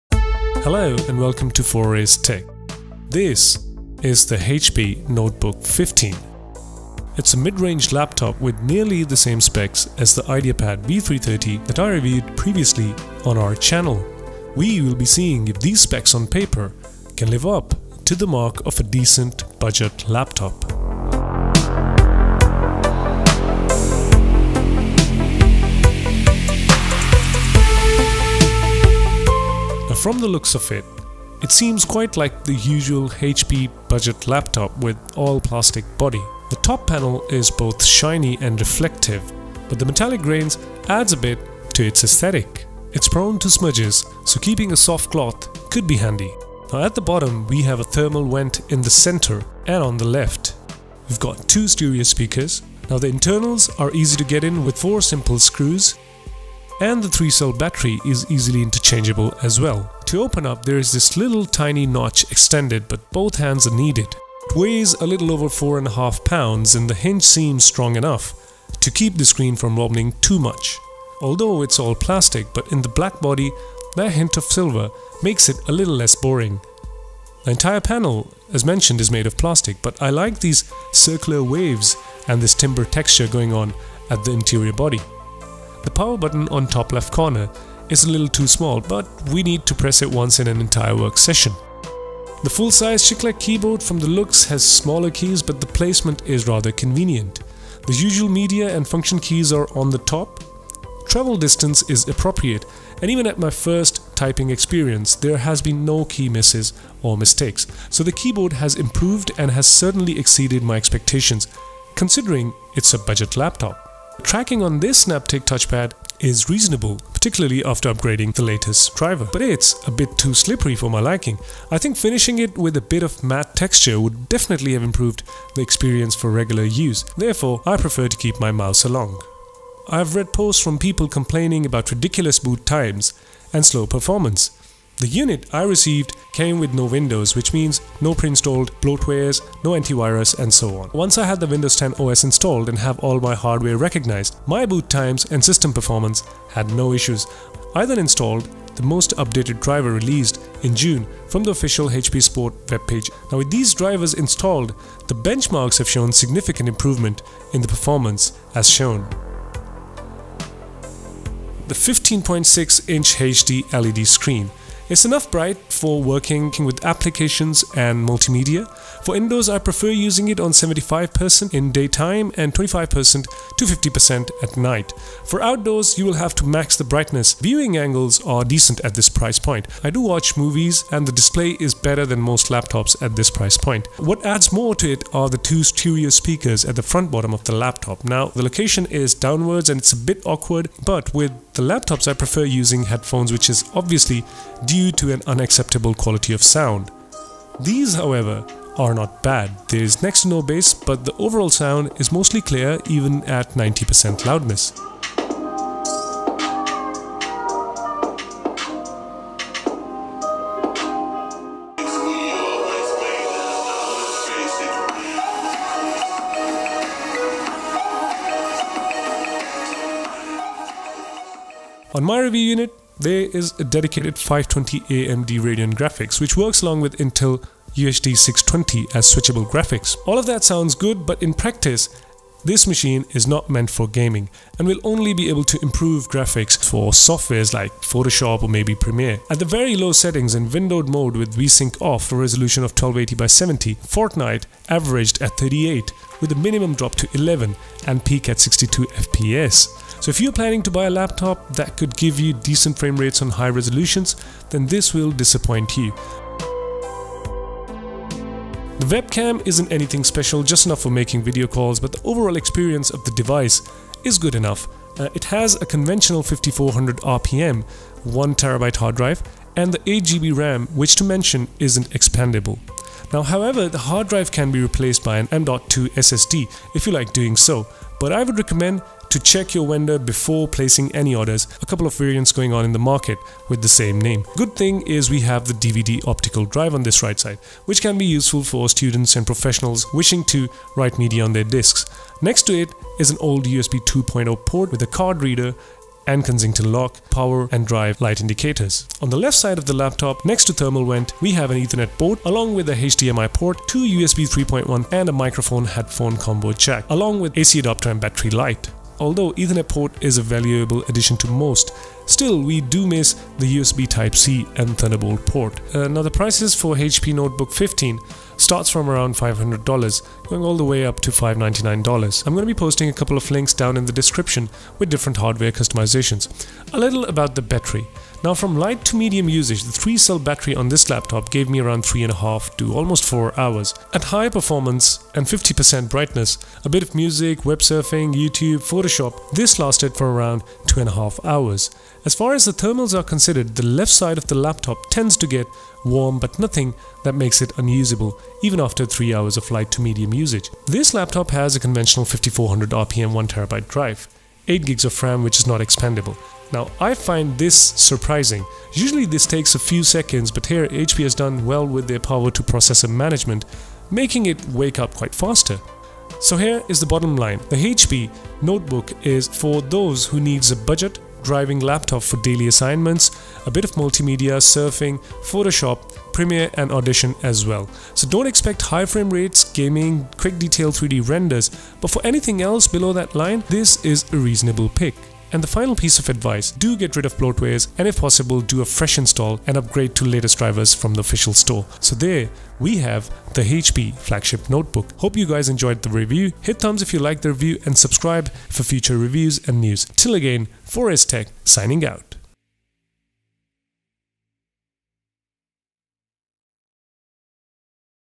the notebook 15 - review 2018
The notebook 15 by HP is it worth buying 2018

Coming with i5-8250U 8th Generation processor, 8GB of RAM, Radeon 520 Dedicated graphics, 1TB storage, HD LED screen & a DVD rewriter inside the HP Notebook 15, makes it a reasonable budget competitor for its price range starting from 500$. The in-depth review brings key aspects to help you decide if its for your ot not. It is a great option for students and professionals on budget.

Thanks.